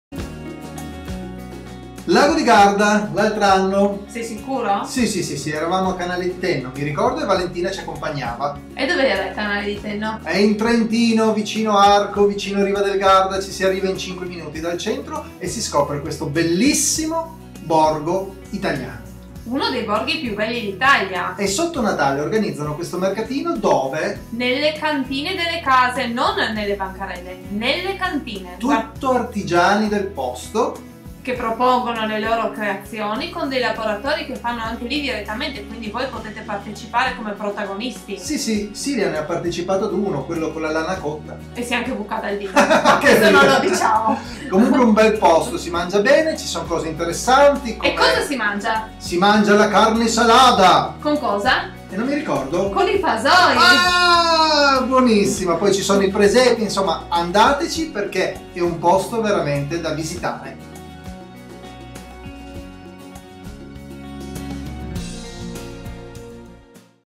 Mercatino di Natale a Canale di Tenno in Trentino
L'altro anno siamo stati a Canale di Tenno In Trentino per il Mercatino di Natale facendoci stupire dalla bellezza ed originalità del luogo!